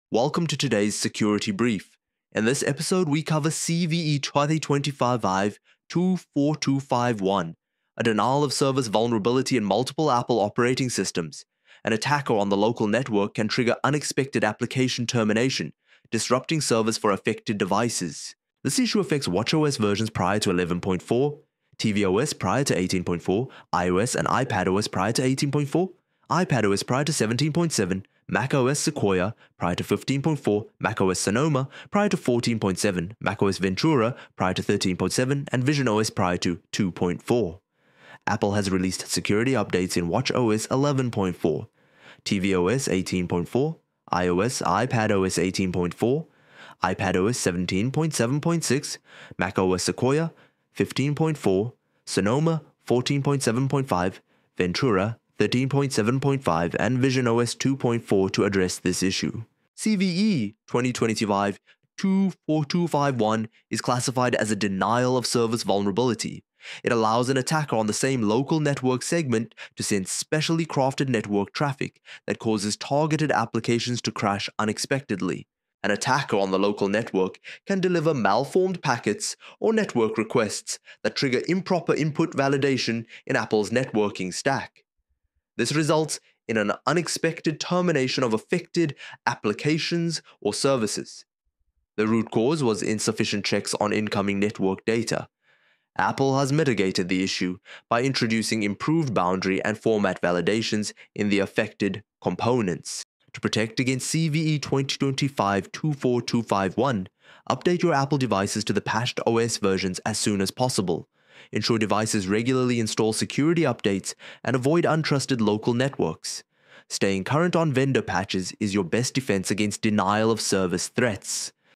Apple OS: local network denial of service in Apple operating systems causing u...(CVE-2025-24251)
A short video explaining the security vulnerability local network denial of service in Apple operating systems causing unexpected app termination and the associated CVE CVE-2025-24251.

00:00 Summary
00:19 Product details
00:60 Vulnerability type summary
01:16 Details of the vulnerability
01:43 Conclusion